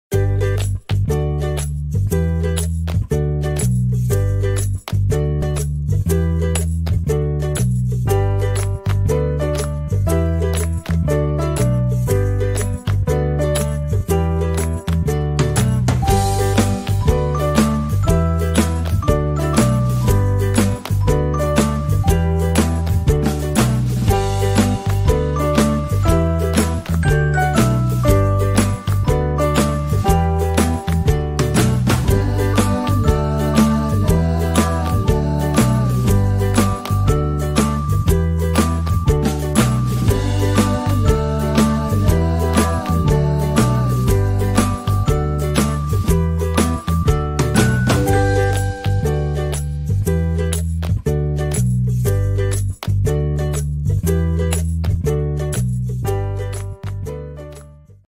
What are the Five reasons Dealers change their Dealer Management System ?
There are dozens, but these are the top 5 we come across !